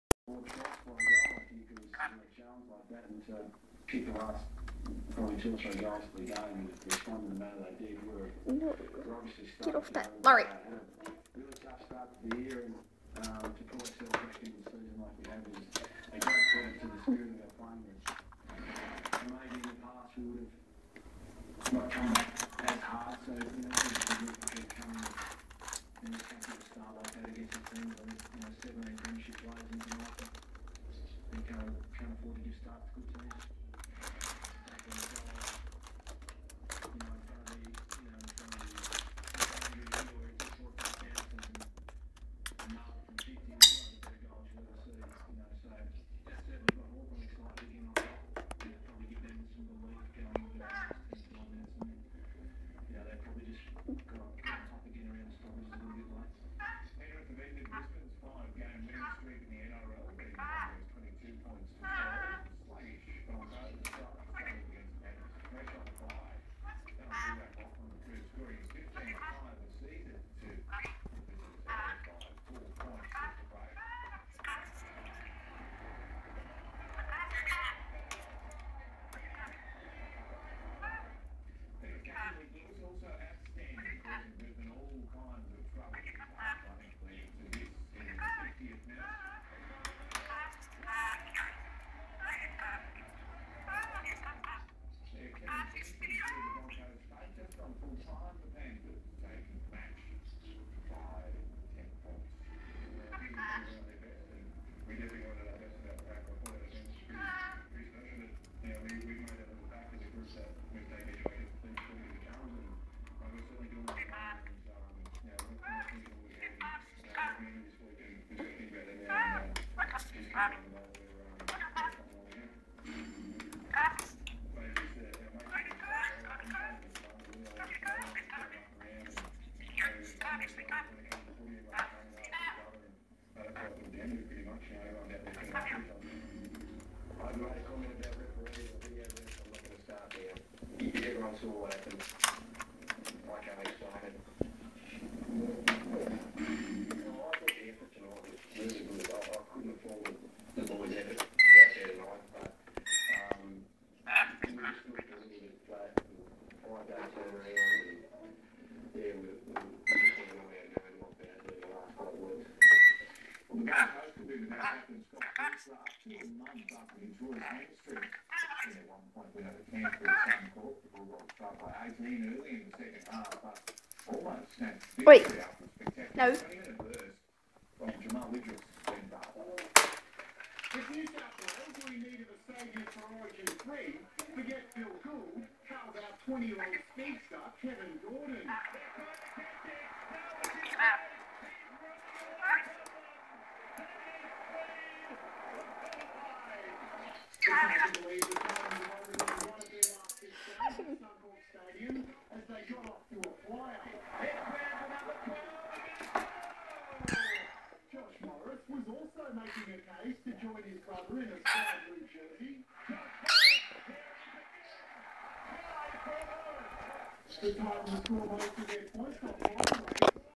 Rainbow Lorikeet Playing on Table
rainbow lorikeet playing with absolutely everything on the computer table.